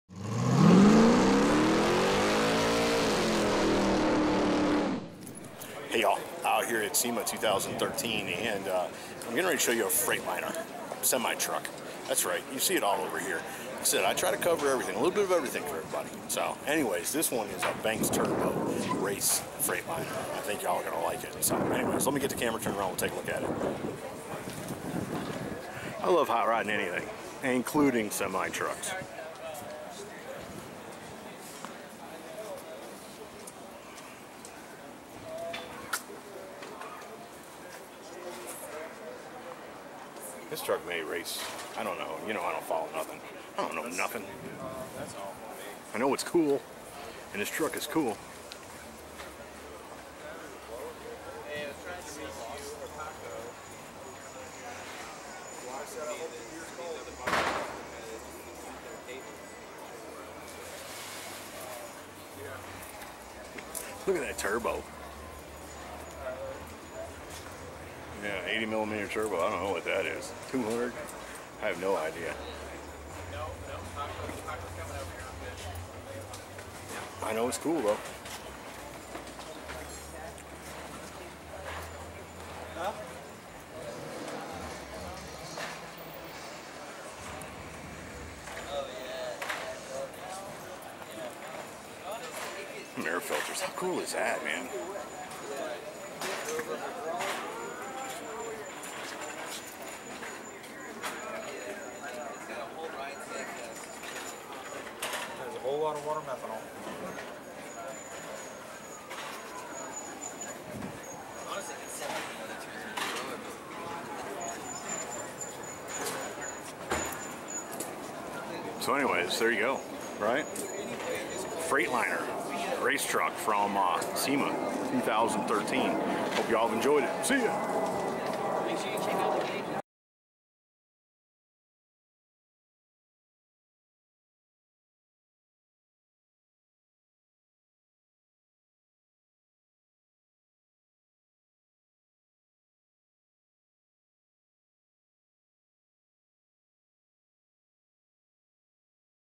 Banks Super-Turbo Freightliner Cascadia SEMA 2013
The Banks Super-Turbo Freightliner Cascadia is a test truck for Banks Power..Compound Boost..A supercharged..Turbo 2800HP Over 5000 lbs torque. I caught up with it a SEMA 2013...Very cool...Check it out!!!,,,Make sure you follow me so that you don't miss any of the awesome videos I post daily!!!
Check it out!!